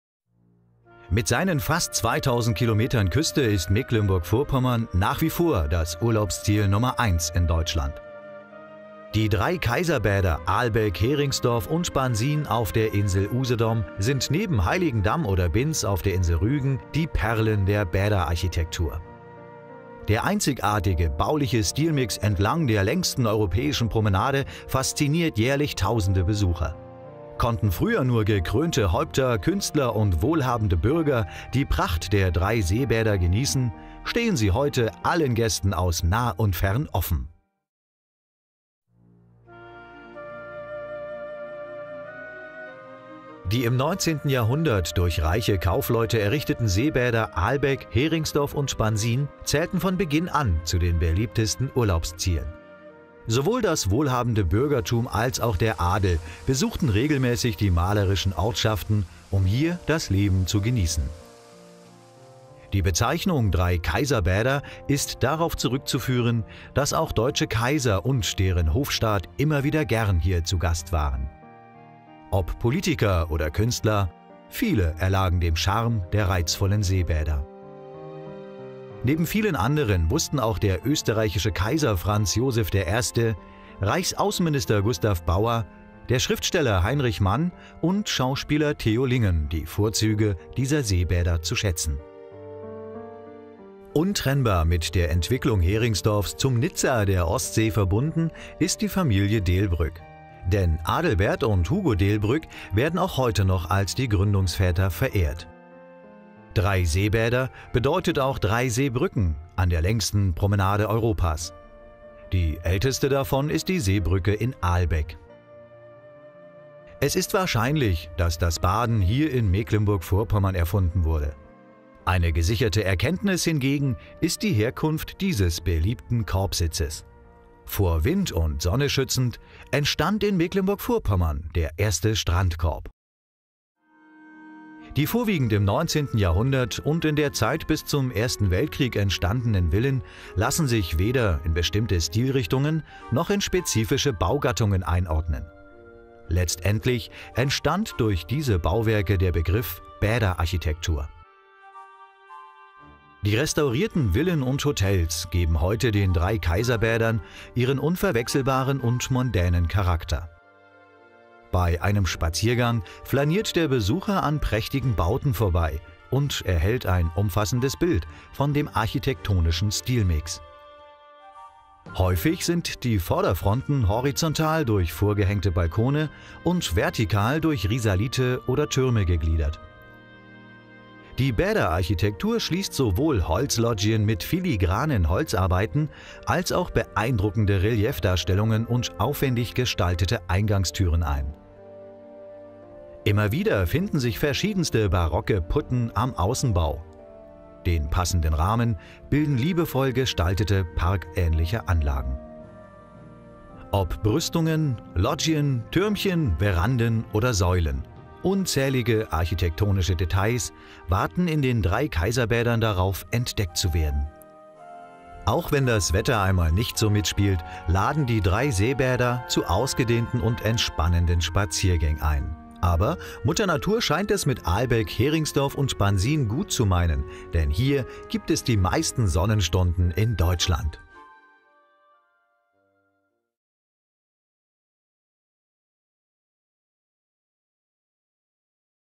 Drei-Kaiserbäder, Seebrücken, Strandkorb
Ein beliebtes Sommerreiseziel.

Die Bezeichnung Drei-Kaiserbäder ist darauf zurück zu führen, dass auch deutsche Kaiser und deren Hofstaat immer wieder gern zu Gast hier waren. Heute erlegen die Besucher dem Charme der reizvollen Seebäder und dem Reiz des Bundeslandes.